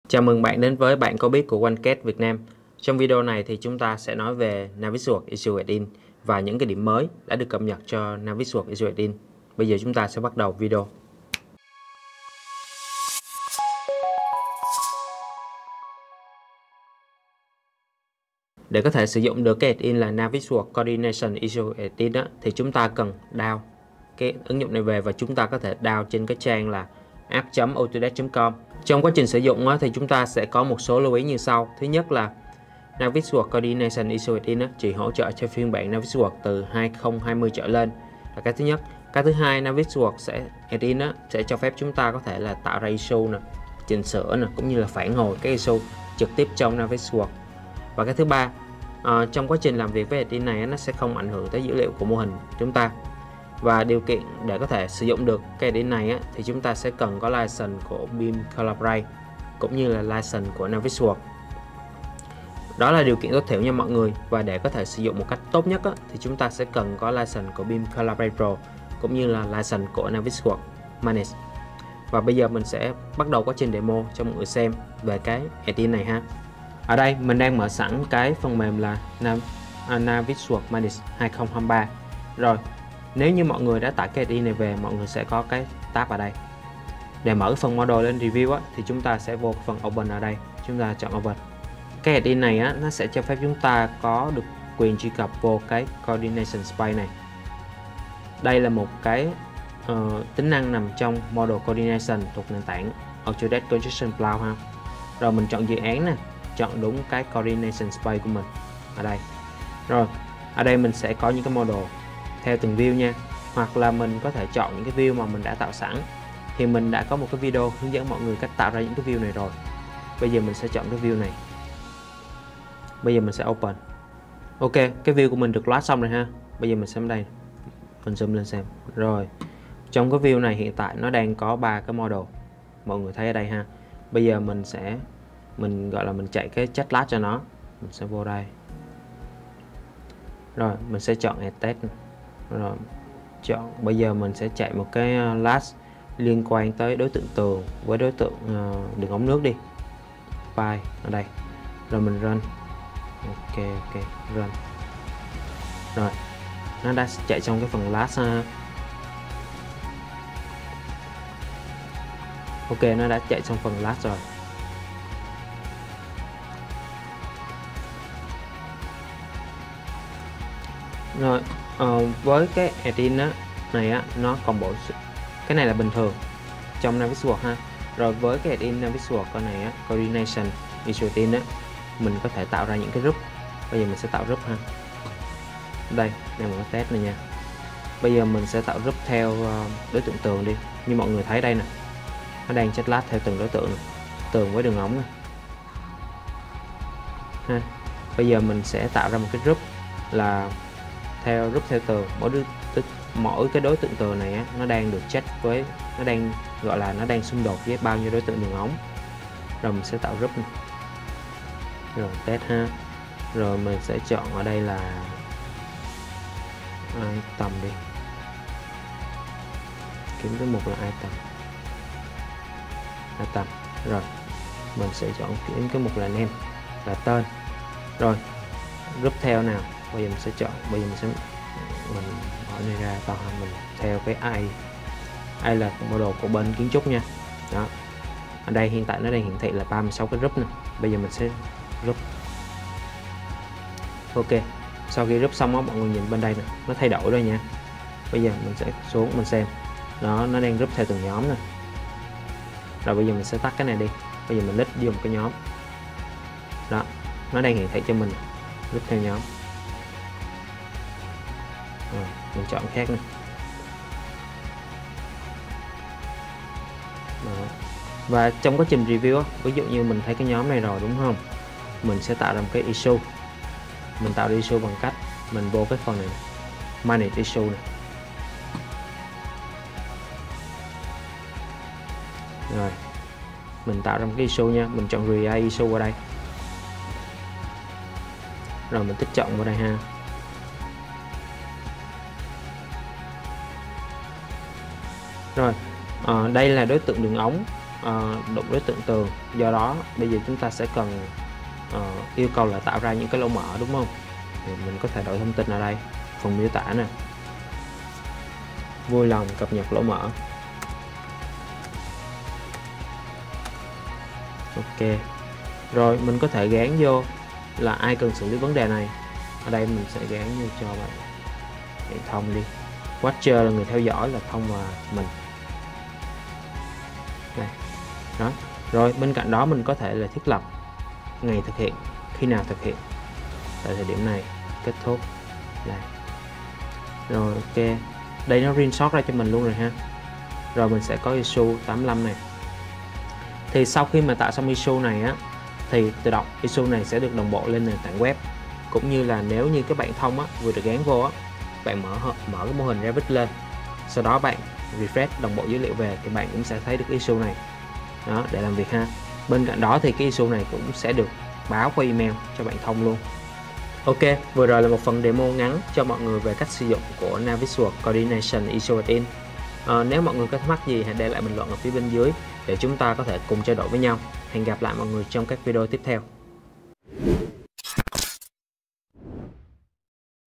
OneCAD I Bạn có biết I Hướng dẫn sử dụng Naviswork issue add in
Naviswork Coordination issue add-in là một ứng dụng không thể thiếu cho người dùng sở hữu license BIM Collaborate Pro và Navisworks Manage. Ứng dụng này sẽ giúp kết nối quy trình làm việc trên nền tảng Autodesk Construction Cloud với phần mềm Navisworks Manage. Chúng ta hãy cùng xem qua video hướng dẫn này nhé.